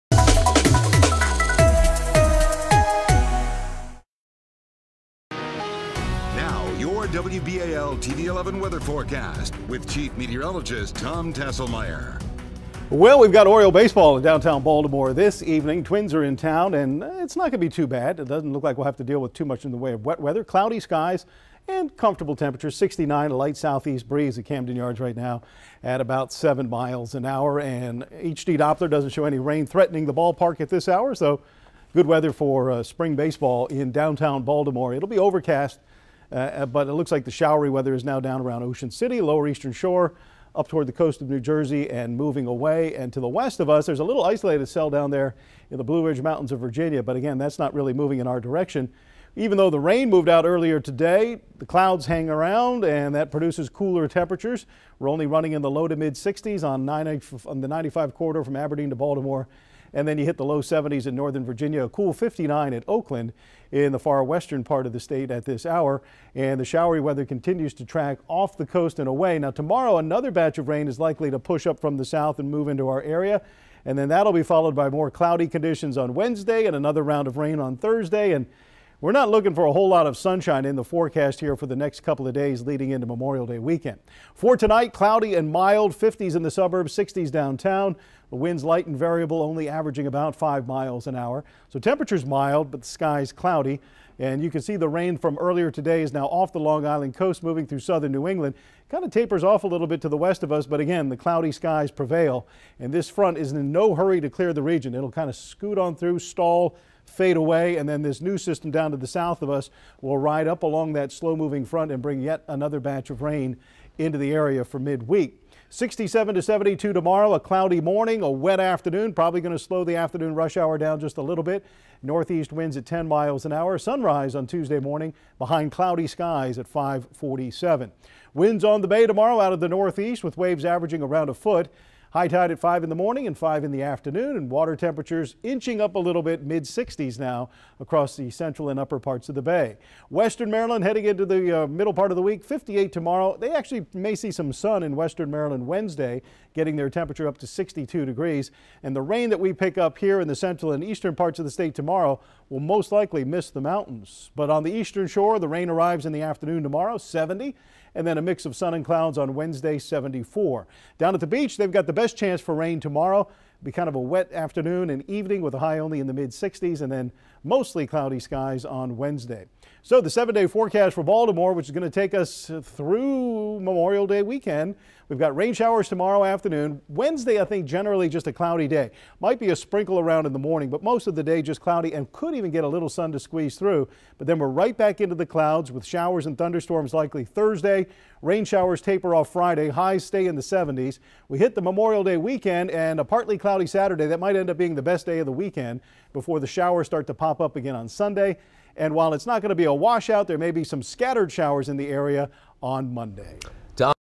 Cloudy, mild conditions with temps in 60s Monday evening
During the Monday evening weather forecast, Tom says we can expect cloudy and mild conditions with temperatures in the 60s.